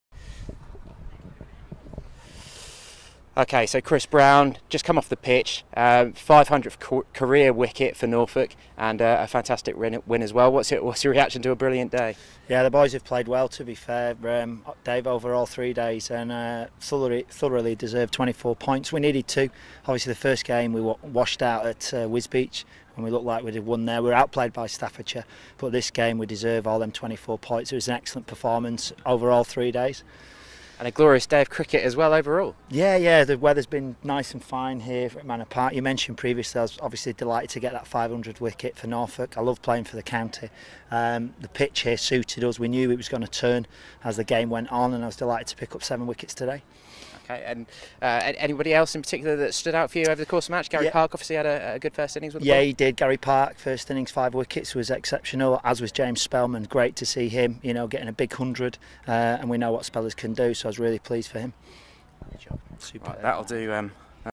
Norfolk captain Chris Brown discusses win over Buckinghamshire
Norfolk captain Chris Brown talks to EDP sports reporter David Freezer following his side's 239-run win over Buckinghamshire in the Unicorns Minor Counties Championship at Manor Park on Tuesday, July 19, 2016. Brown also took the 500th wicket of his Norfolk career in the match.